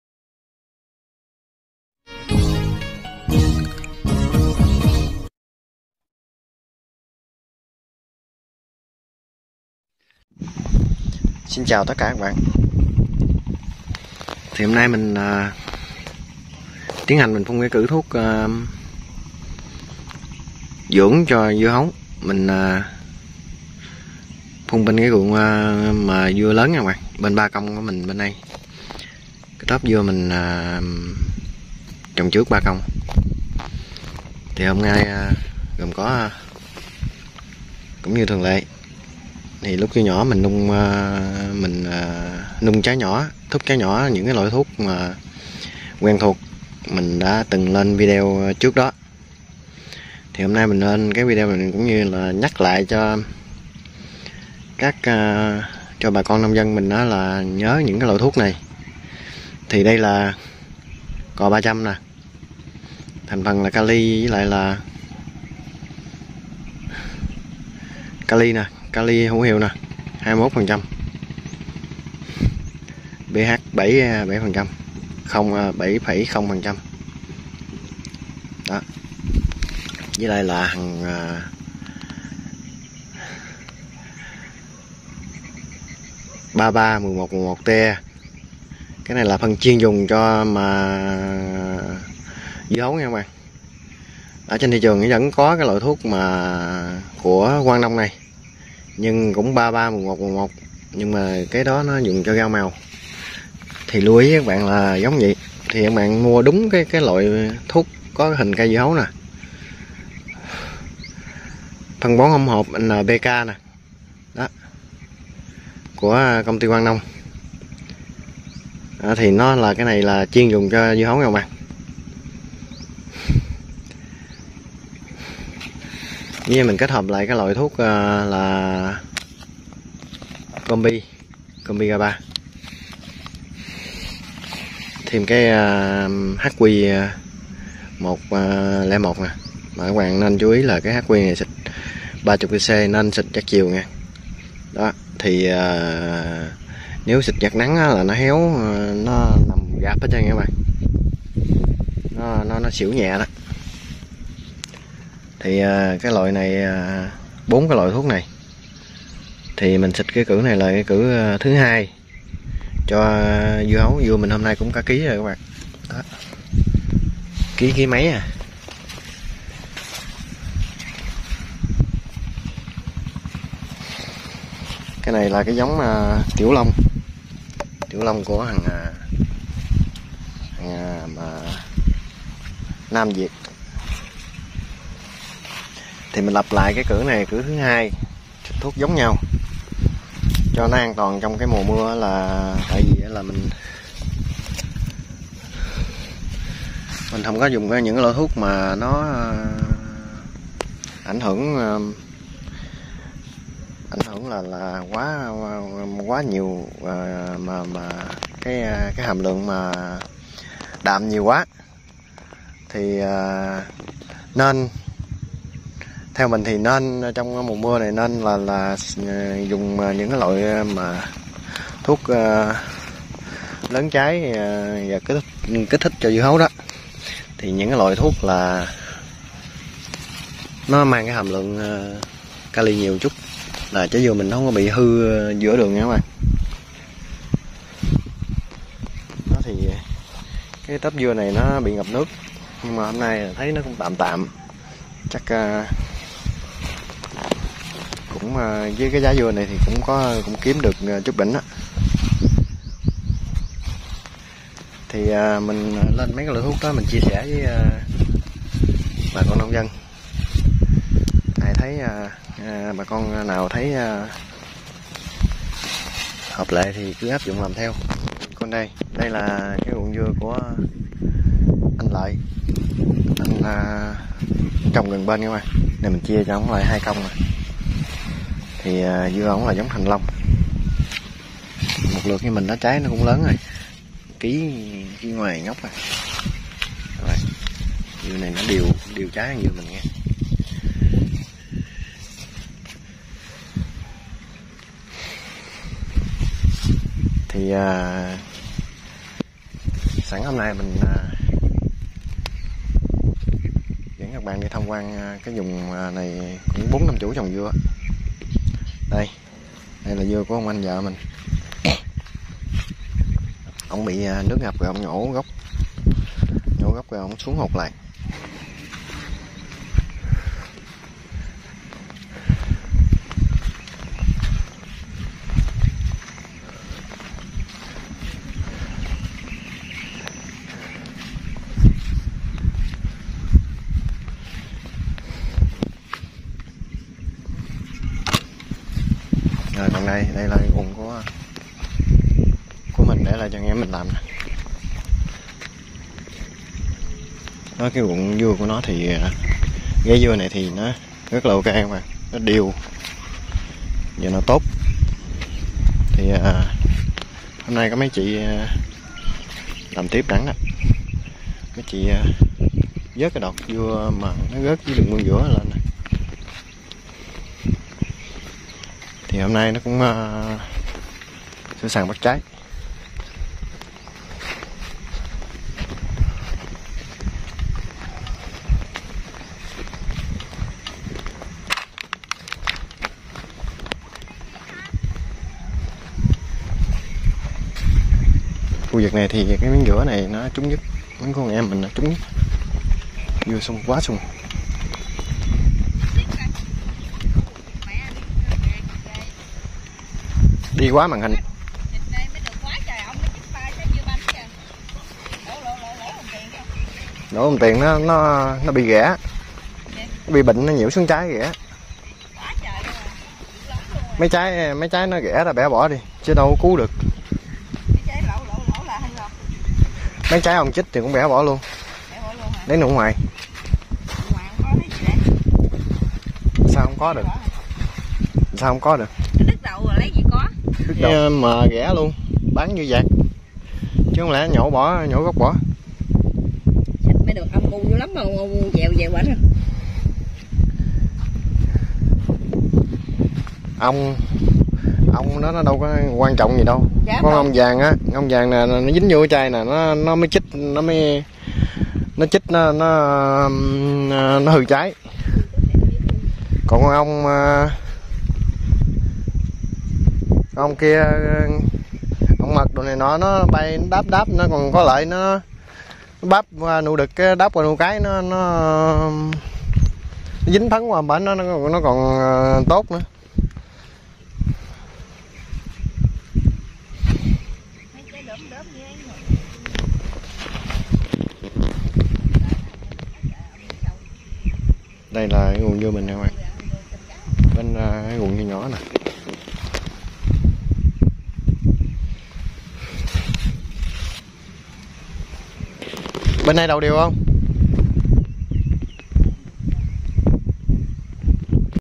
kỹ thuật trồng dưa hấu thuốc dưỡng dưa hấu trong mùa mưa
kỹ thuật trồng dưa hấu cử thuốc dưỡng trong mùa mưa bà con nông dân trồng dưa hấu nhớ lưu ý dùng thuốc dưỡng trong mùa mưa ☔ sẽ có lợi bất cập hại nhớ nhé chúc bà con nông dân một mùa bội thu